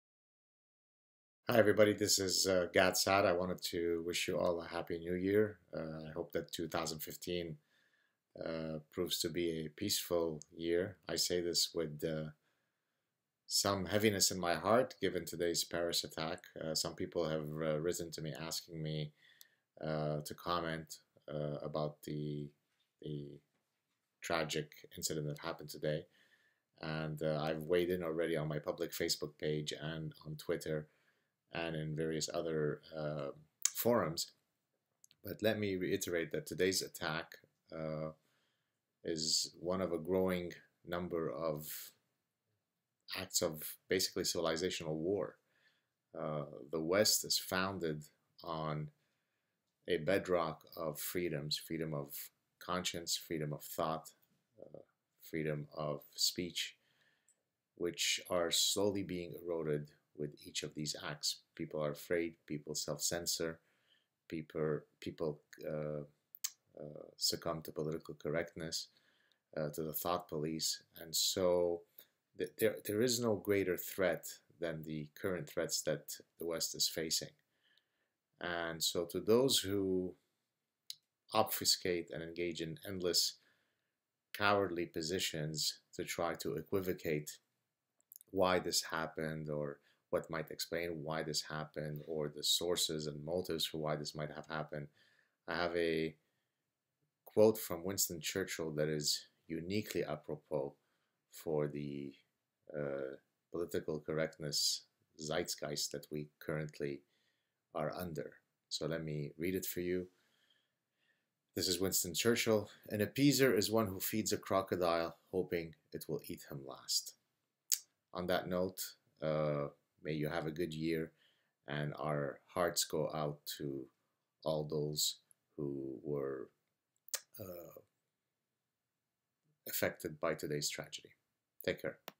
THE SAAD TRUTH_22: The Paris Terror Attack
A society that does not fight for its defining ethos is not worth its salt. This attack and others like it undermine the foundational bedrock of Western freedoms.

Lighting Issue:  I noticed that there is a shadow around my right eye that makes it look as though I have a black eye!